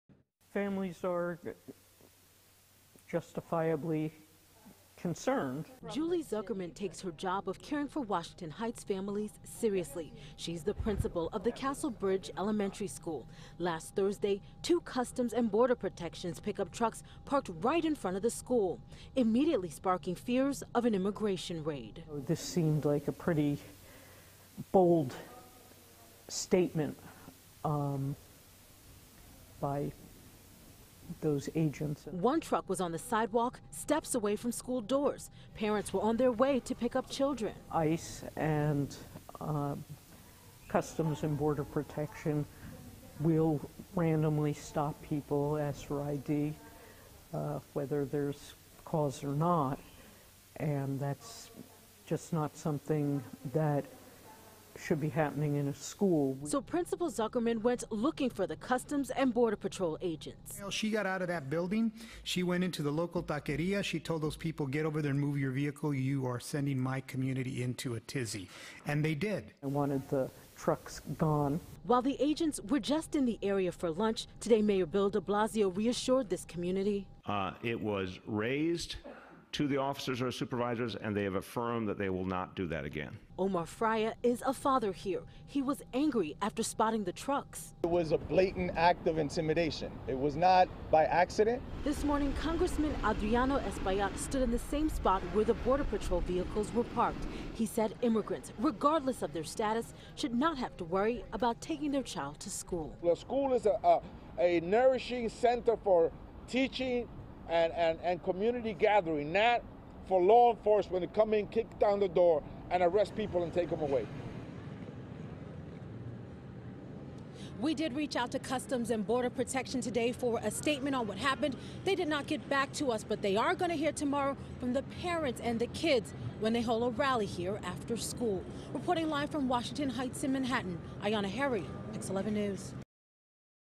Immigration trucks parked outside Bronx school cause panic, outrage
Castle Bridge School principal Julie Zuckerman said she was alerted by a parent and asked the agents to move.